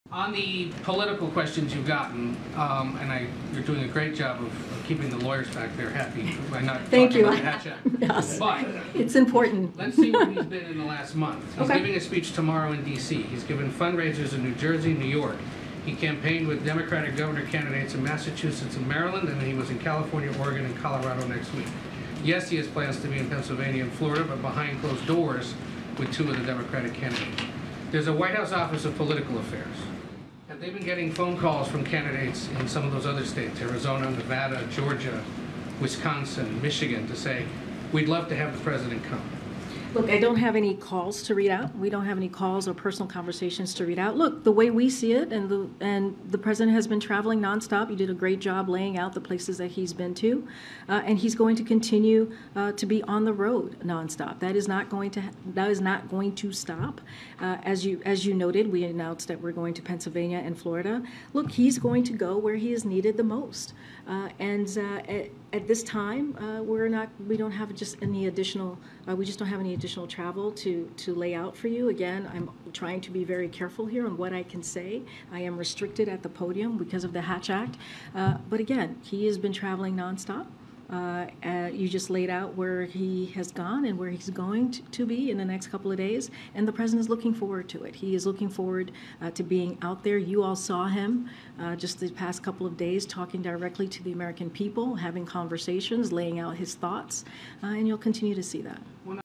Reporter Tells Karine Jean-Pierre She's Doing A ‘Great Job Of Keeping The Lawyers Back There Happy’
During a White House Press Briefing on Monday, Karine Jean-Pierre spoke about President Biden supporting candidates on the campaign trail.